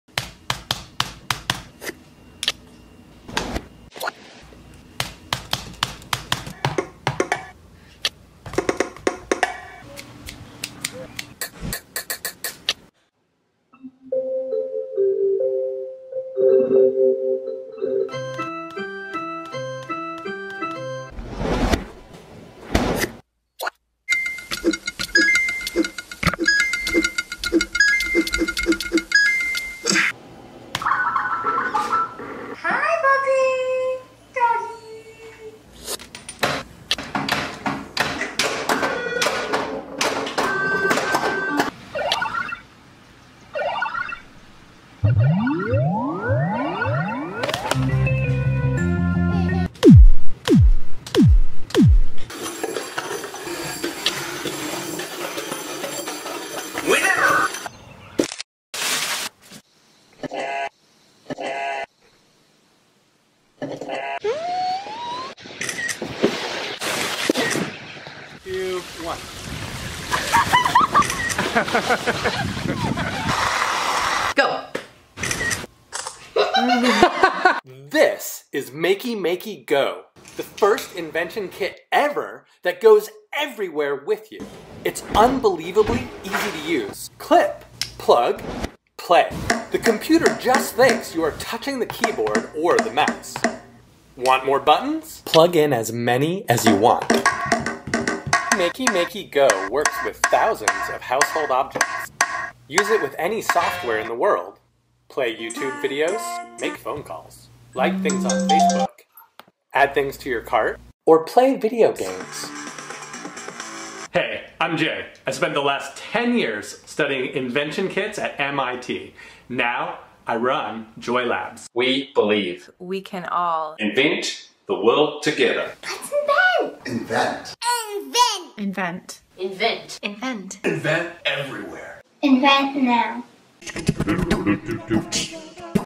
Makey Makey GO: Invent Everywhere, Invent Now!
Makey Makey GO is the World’s First Invention Kit that goes with you everywhere. Makey Makey GO catalyzes the same creativity and ingenuity in creators as the original, award-winning Makey Makey Classic and gives today’s tinkerers, dreamers, and makers even more mobility—allowing them to invent anytime, anywhere.
Small enough to fit on your keychain, book bag, or bracelet, Makey Makey GO turns everyday objects into touchpads and combines them with the internet. It’s a simple tool-slash-toy that allows beginners and experts to make countless art, music, engineering, and science projects. It comes ready to use out of the box with no setup and no installations. Just plug and play.